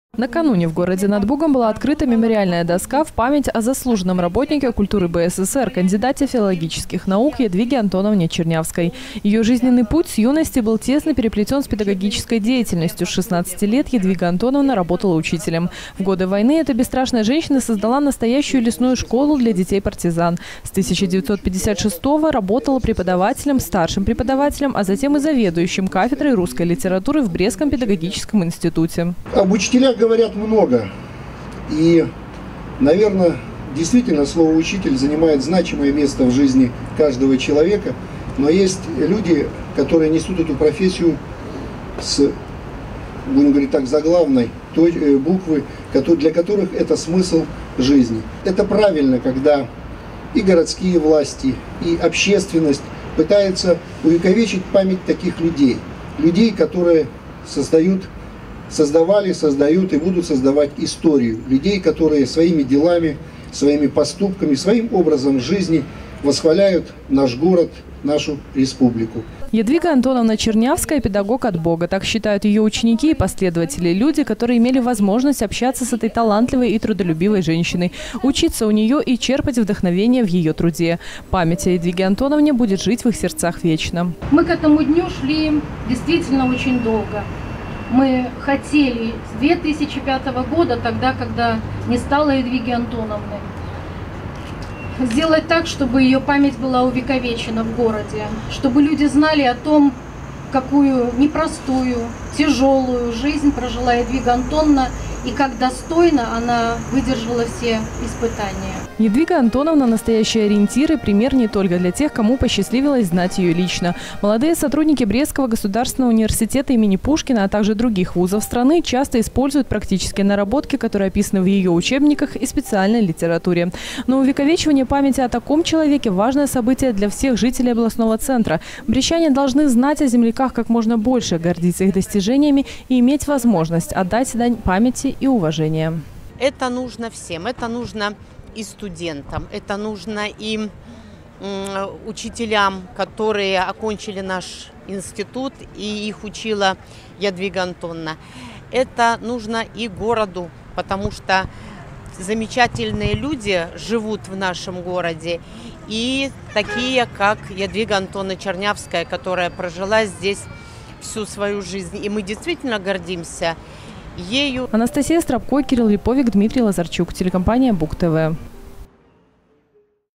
2014-11-12 г. Брест. Открытие мемориальной доски в память об Я.А. Чернявской. Телекомпания Буг-ТВ.
Классик русской и мировой литературы Лев Толстой когда-то сказал: «Не тот учитель, кто получает воспитание и образование учителя, а тот, у кого есть внутренняя уверенность, что он есть, должен и не может быть иным. Эта уверенность встречается редко и может быть доказана только жертвами, которые человек приносит своему призванию».
Быть может, именно такая уверенность на протяжении всей жизни сопровождала и Ядвигу Антоновну Чернявскую. Талантливого педагога, человека необычной судьбы и удивительную женщину. Ядвига Антоновна прожила длинную жизнь, в которой была и война, и «лесная школа», и множество научных поисков, и, конечно, бессчётное количество друзей – последователей, объединённых чувством уважения к своему наставнику. Многих из них и собрало накануне важное событие – открытие мемориальной доски к столетию со Дня рождения Ядвиги Антоновны.